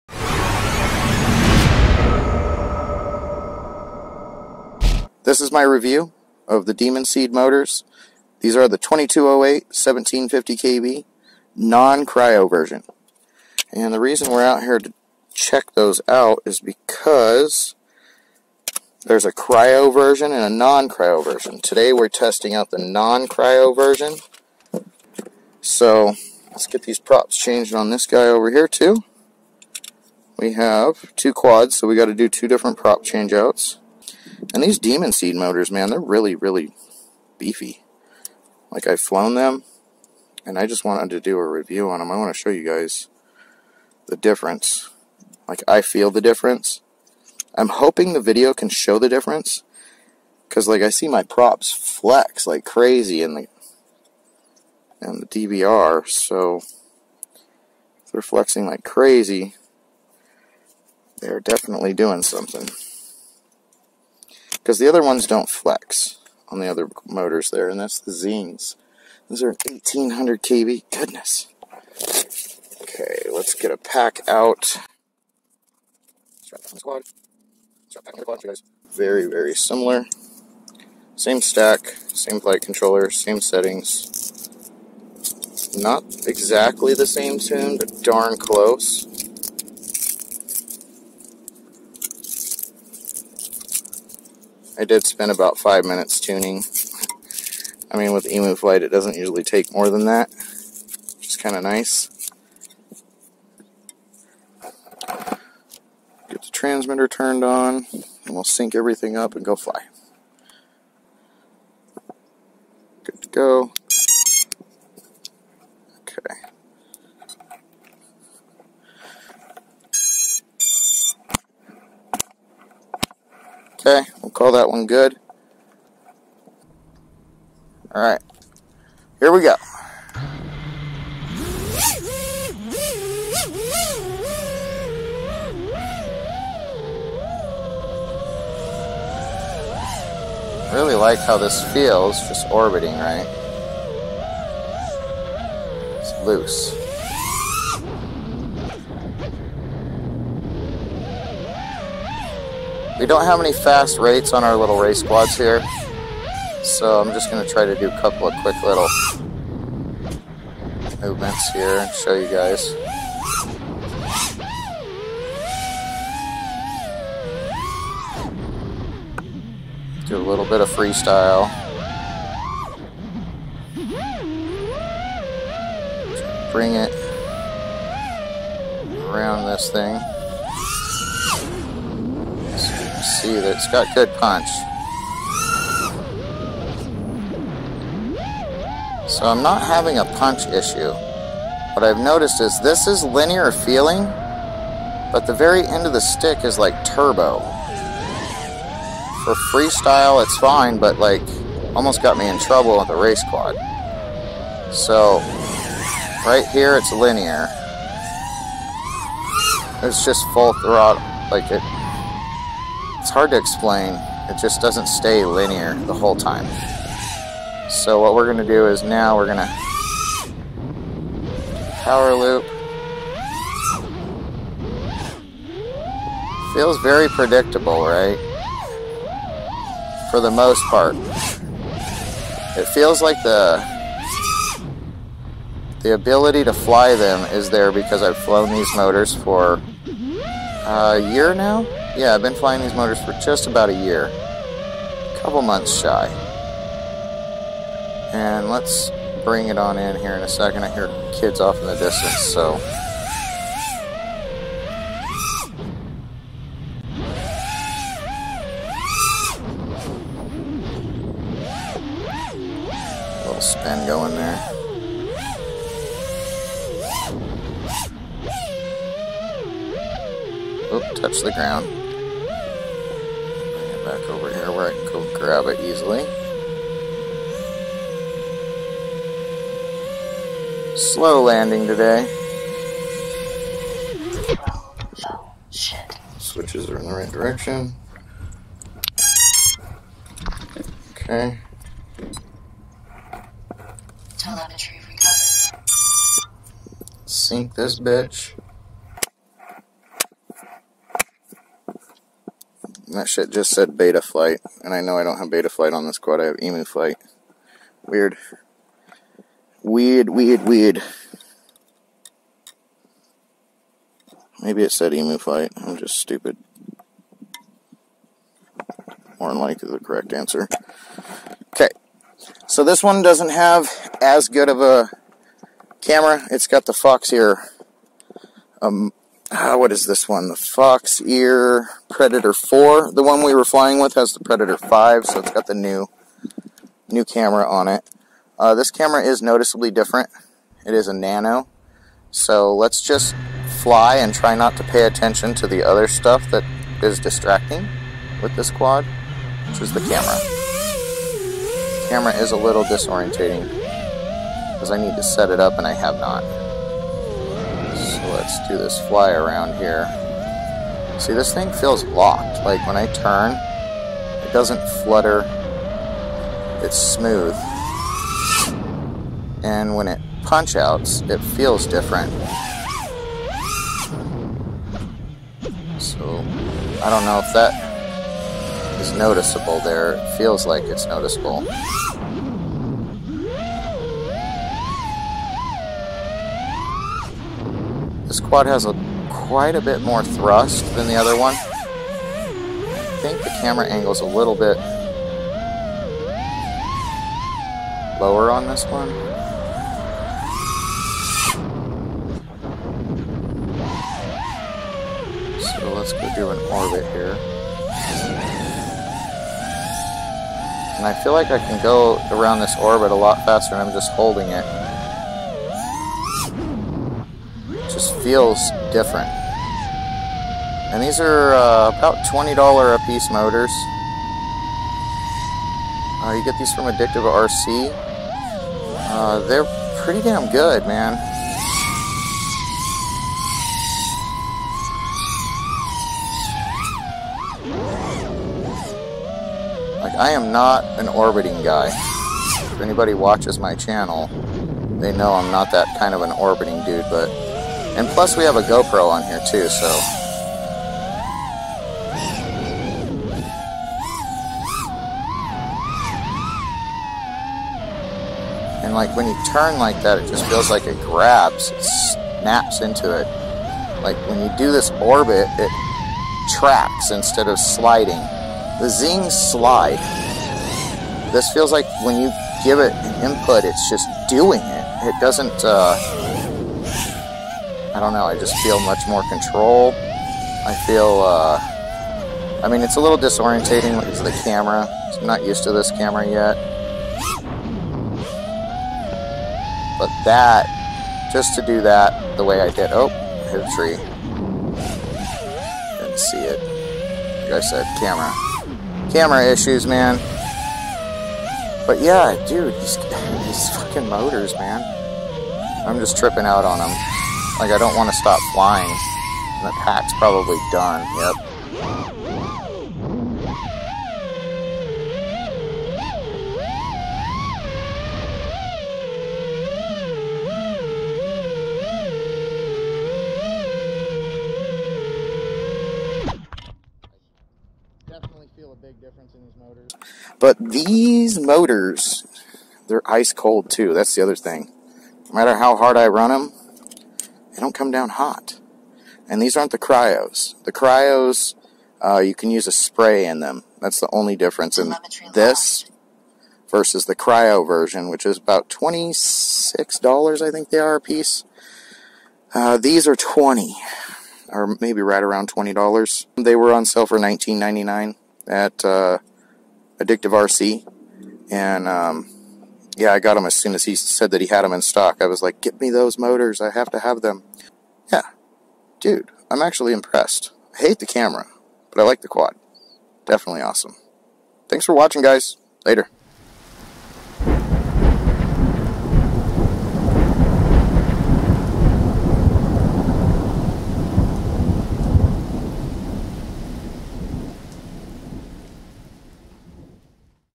Demon Seed Brushless Motors! A non Cryo Version by KO Technologies - Must Watch B4 U Buy for $19.99
-Today We Compare The Demon Seed Motors to a Similar Popular Top Rated Racing Motor-

VTX - ImmersionRC Tramp hv
FPV Camera - Foxeer Predator v4 nano
VTX Antenna - Nidici sma Stubby 5.8

AddictiveRC - US Based Online Aerial Drone and FPV Products Store In Tucson, Arizona